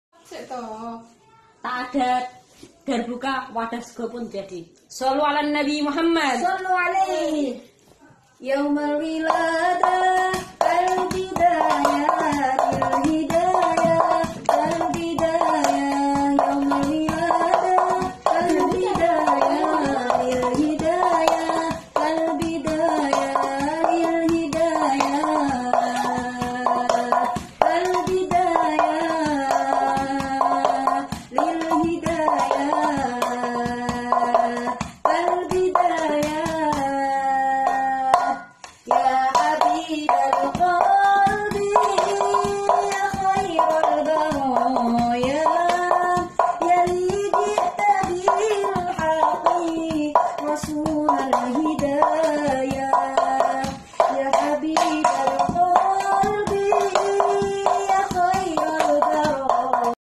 Santri wanita pemain darbuka
Pintar ya memainkan darbuka menggunakan teflon seadanya